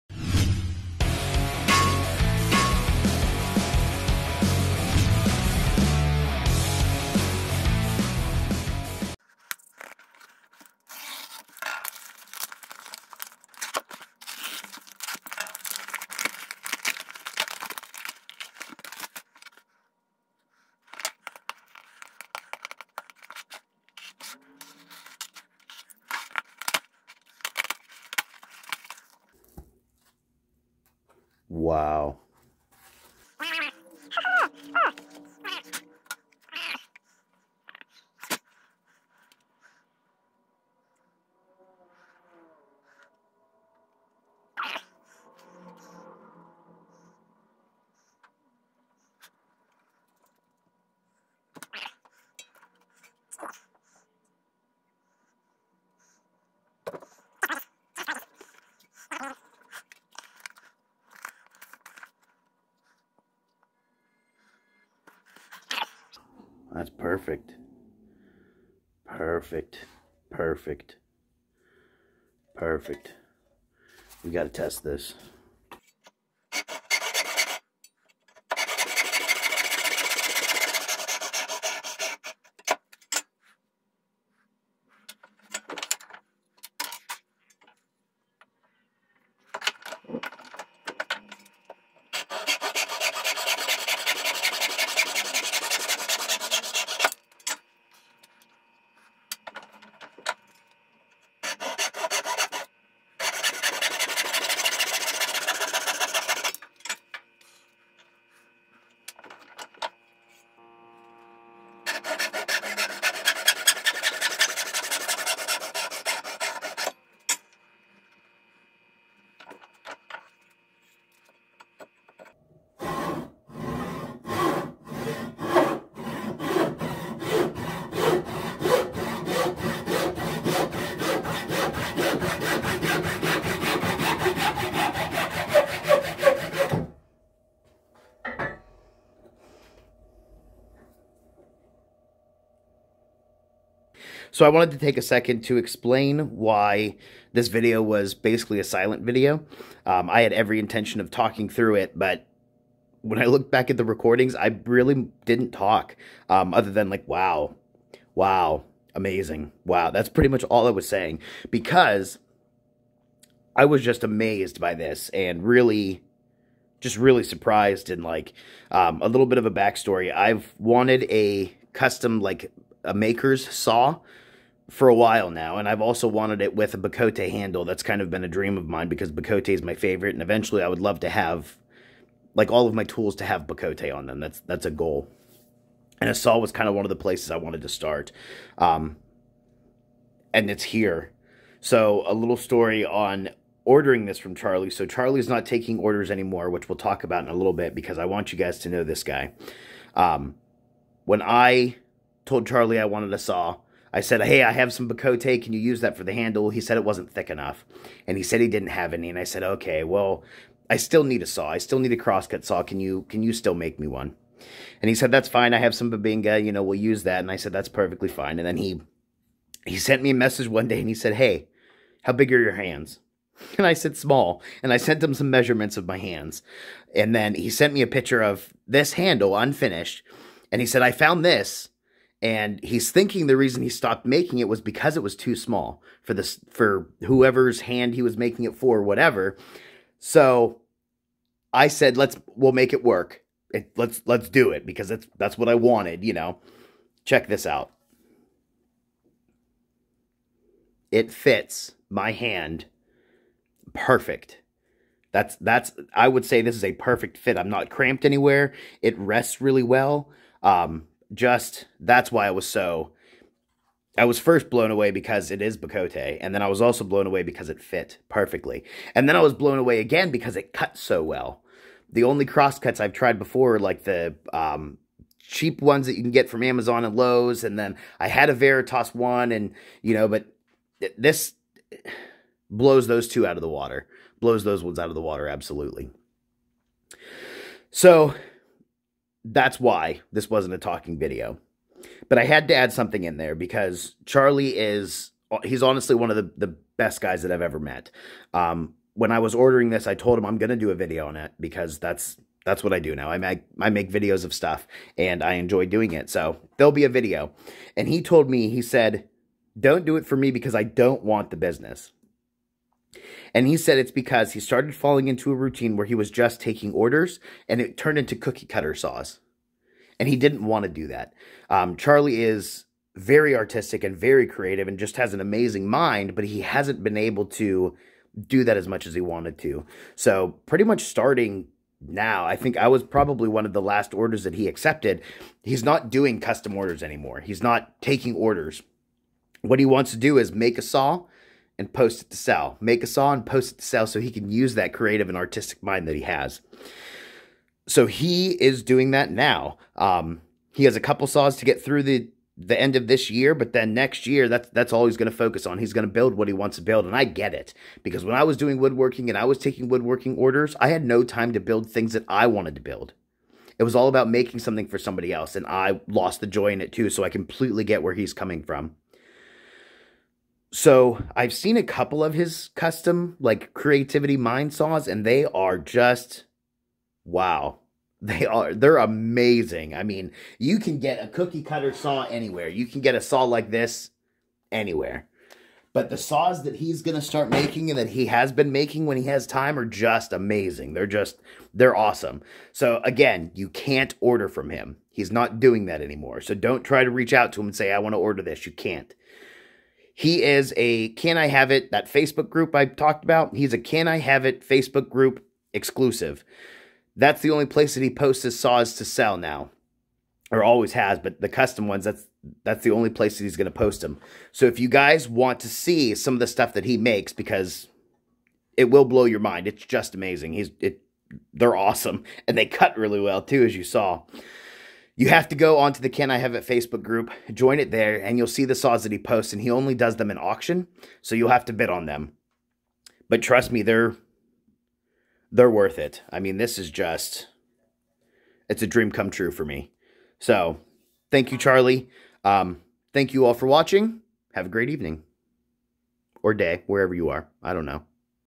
MY BOCOTE DREAM SAW! Spring Green Saw Works Unboxing and Testing - Random and just had to share!
In this video I unbox and test out a saw made by Charlei Earnest - Spring Green Saw Works. He is no longer taking orders because he wants to start making one of a kind custom saws. None of the cookie cutter saws most businesses are making now. The saws he will be making are only available in the Can I Have It Facebook Group. So, if you wanna see some of the amazing saws that he makes join us!